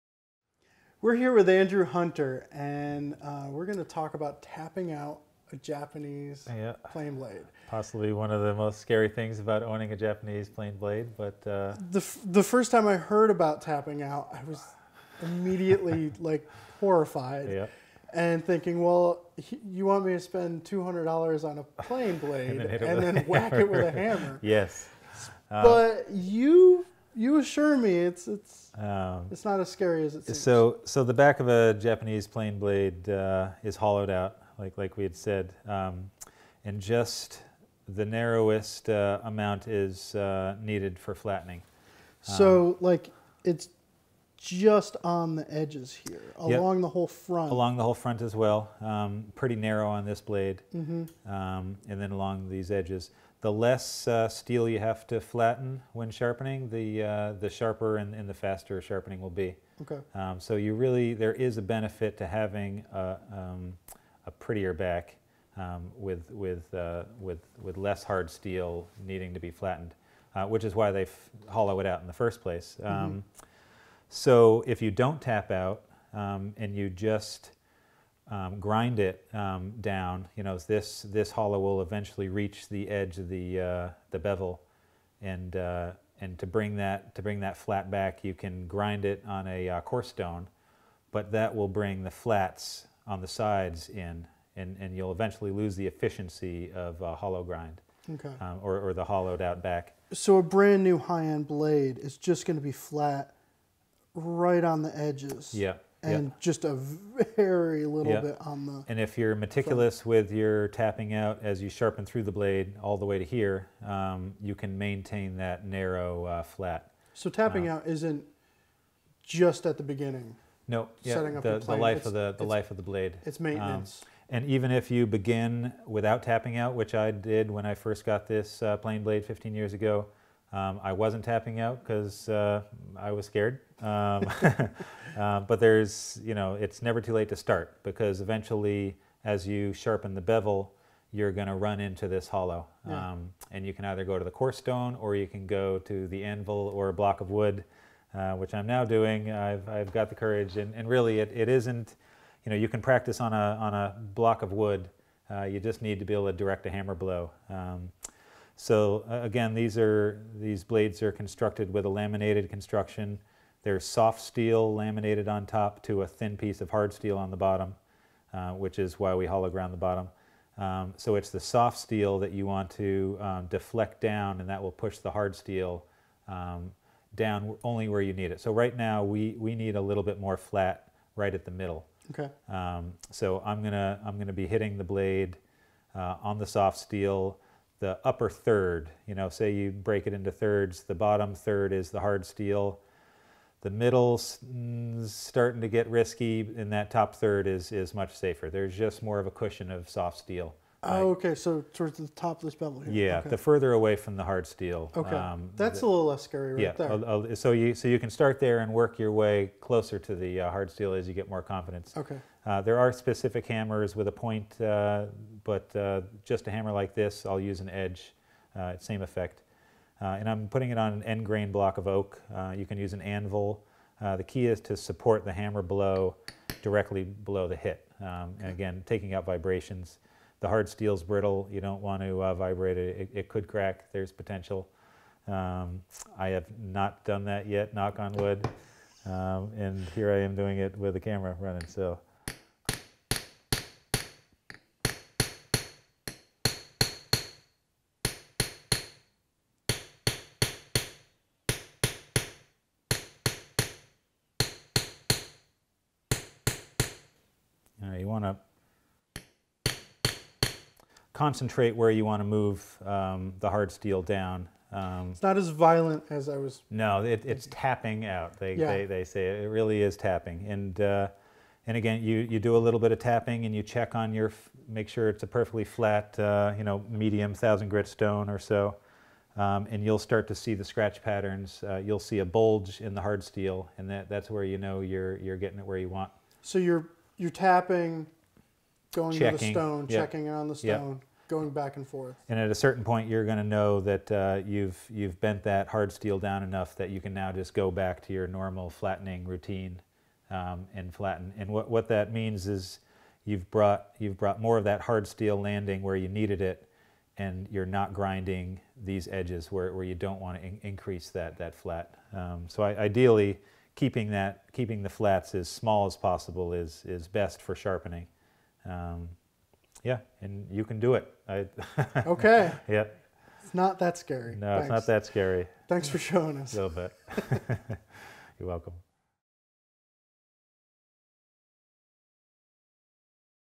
Tapping Out a Japanese Plane Blade with Andrew Hunter
Through repeated sharpening of the bevel, the front flat on the back of a Japanese blade will begin to disappear. The solution is to tap the layer of soft steel above the cutting edge to press the hard steel downward slightly.